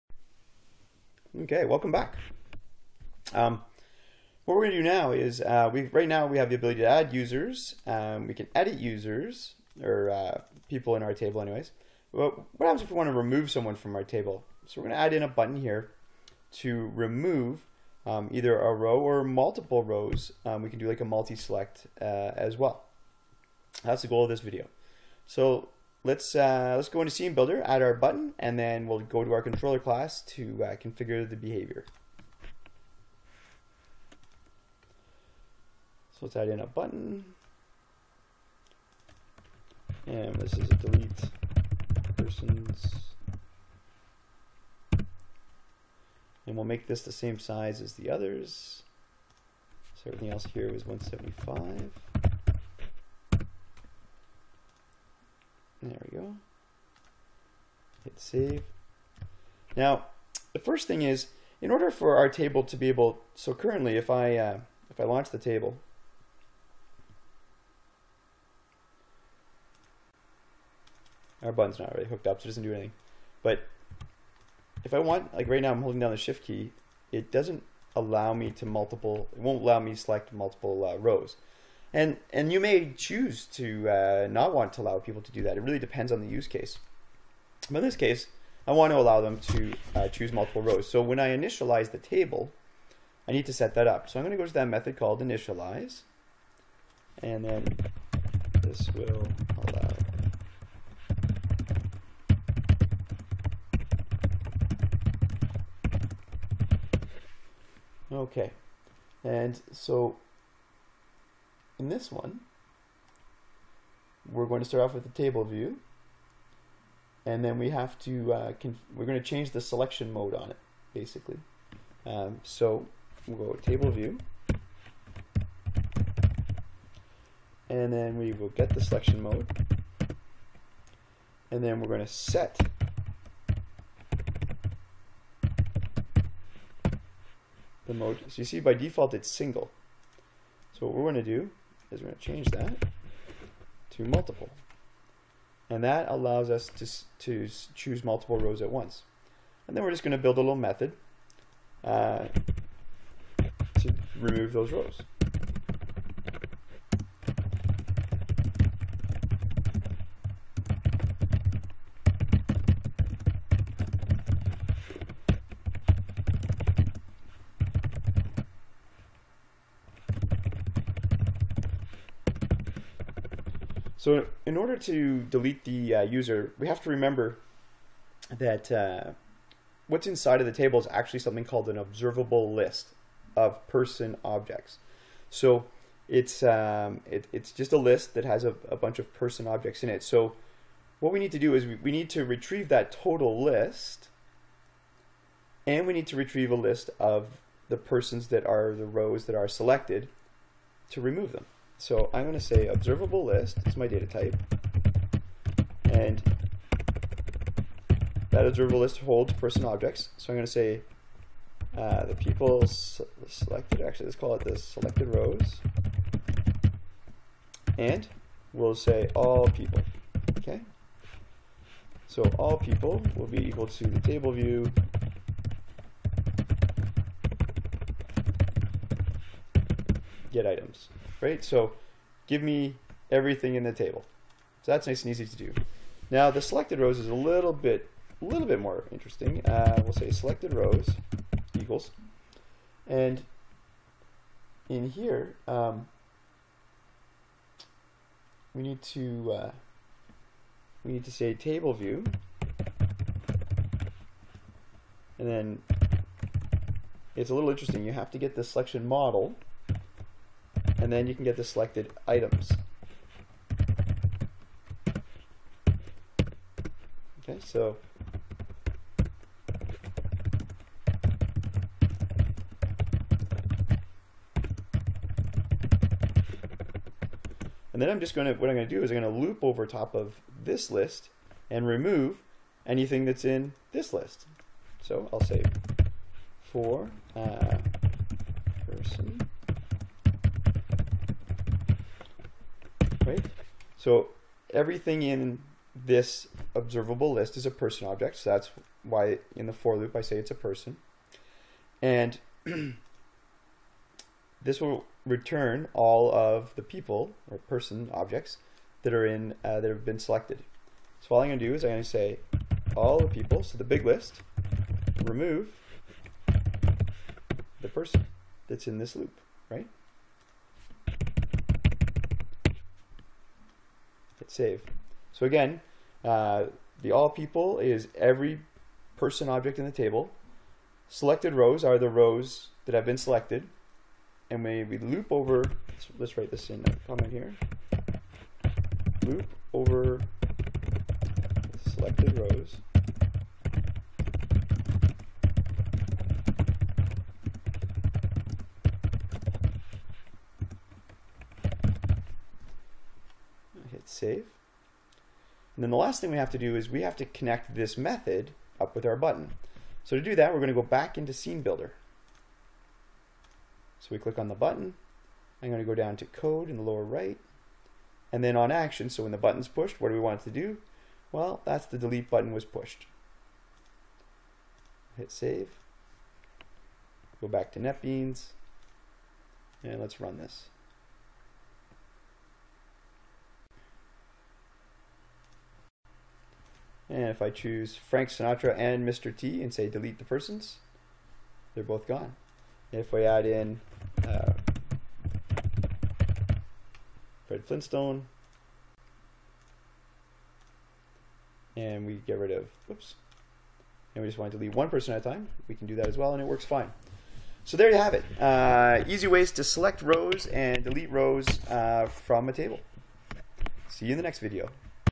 Delete Object from TableView
This video shows how to configure a TableView to support multiple selections and delete objects from a TableView.  It shows configuration of SceneBuilder and the controller class in Netbeans.